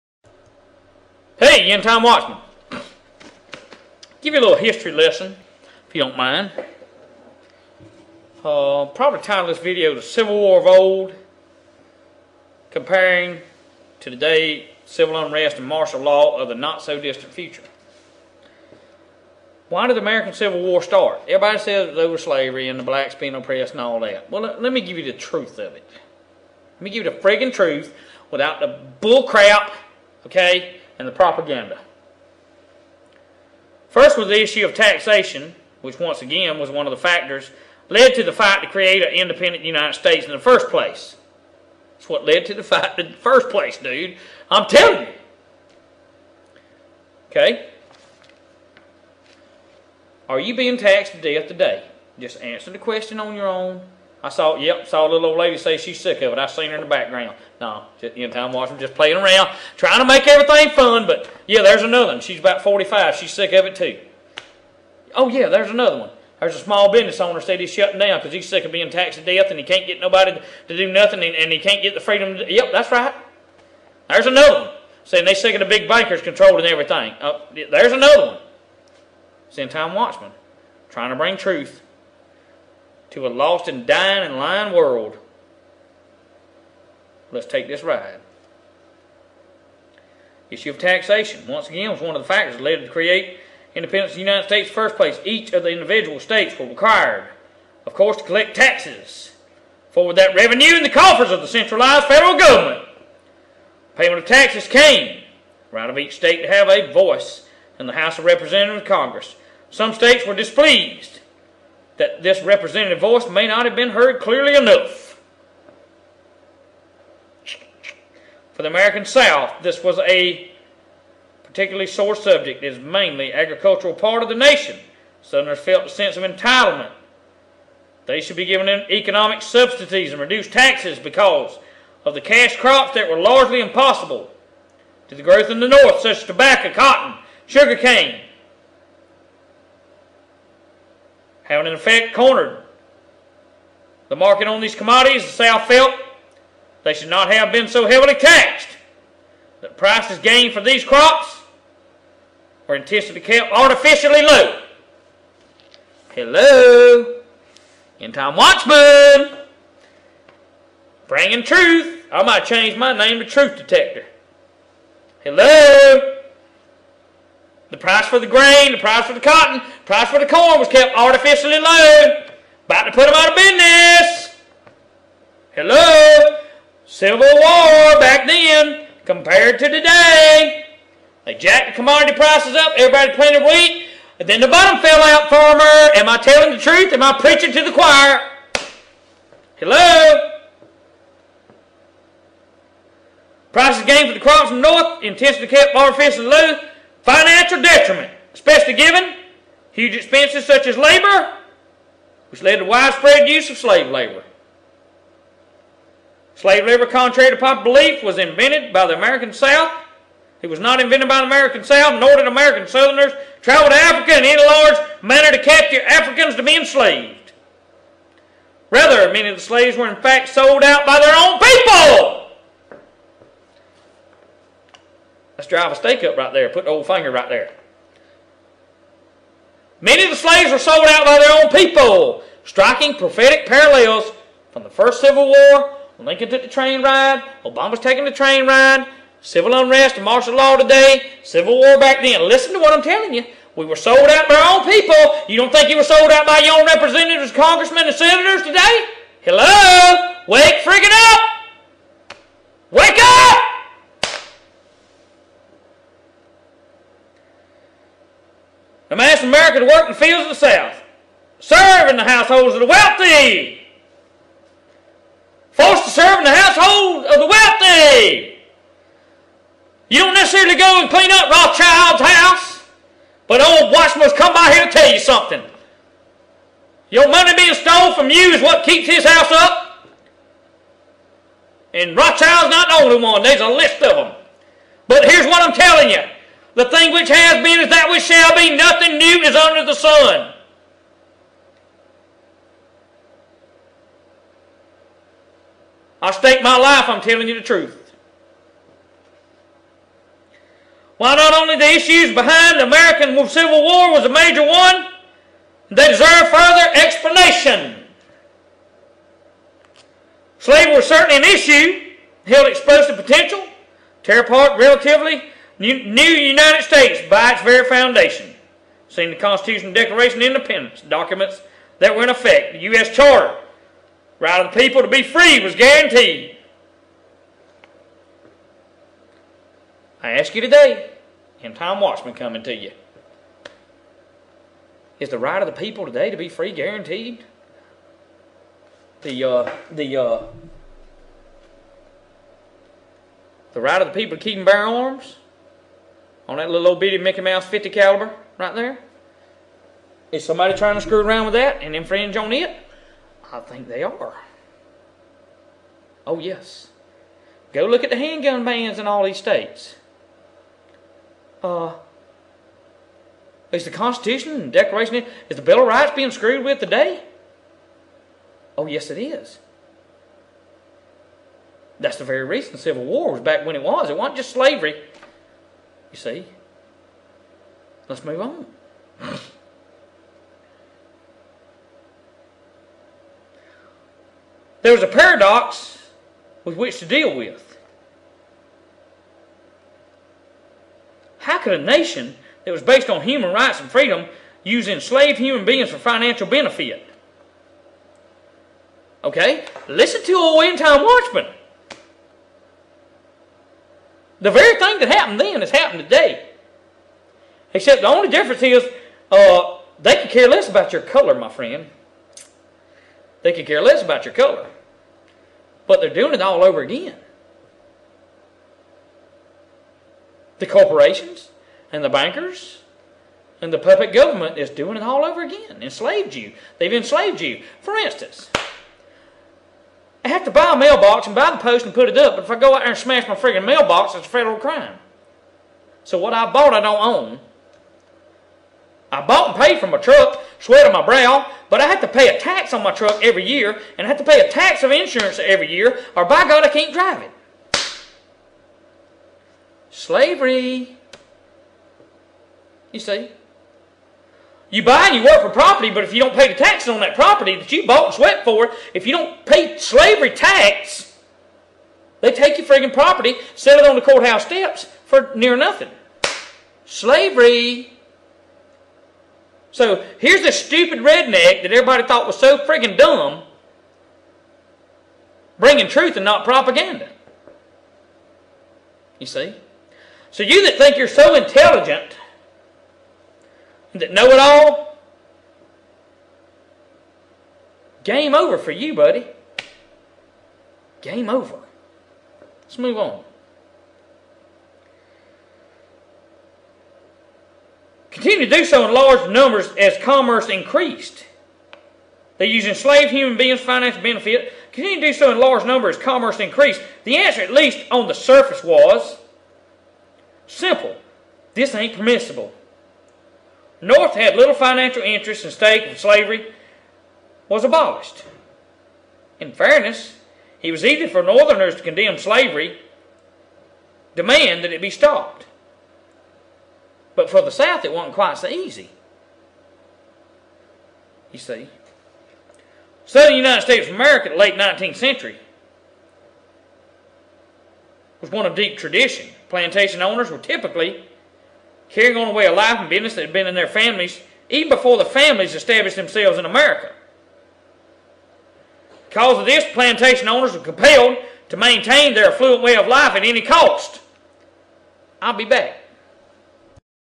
endtimewatchman : Civil War of Old; Civil Unrest; Martial Law of the Near Future - 1 of 2
endtimewatchman reloaded: All videos being reposted on the behalf of endtimewatchman